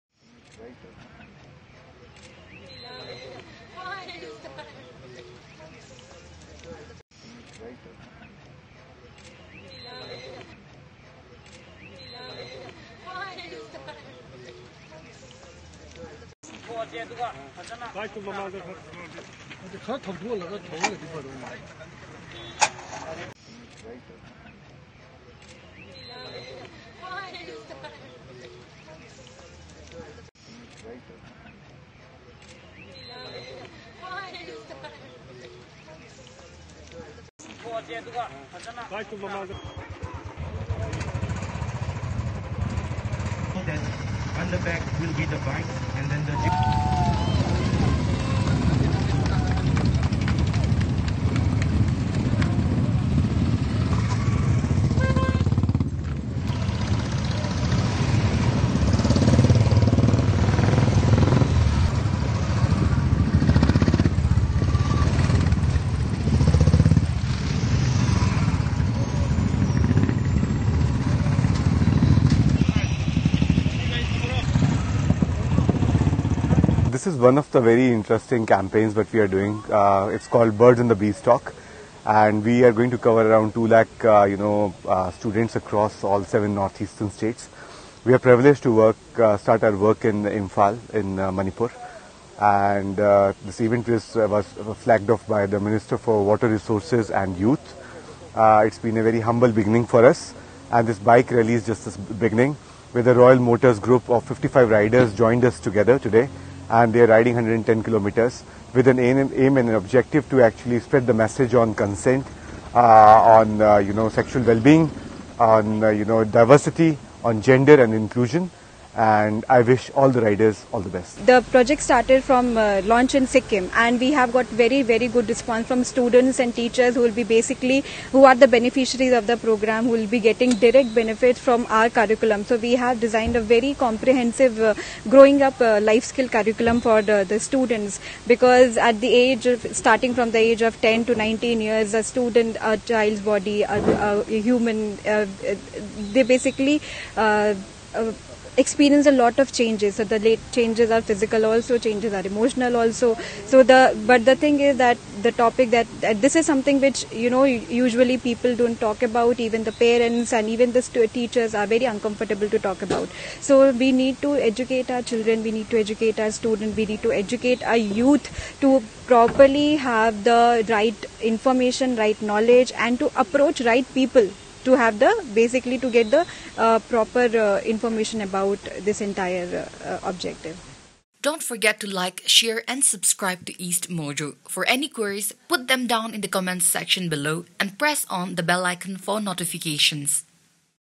Bike rally aimed at spreading awareness on sexual wellbeing kicked off in Imphal
Minister of water resources, youth affairs and sports, Letpao Haokip, on Thursday flagged off a bike rally from Khuman Lampak Sports Stadium in Imphal to show solidarity towards the health and well being of the adolescents in the state.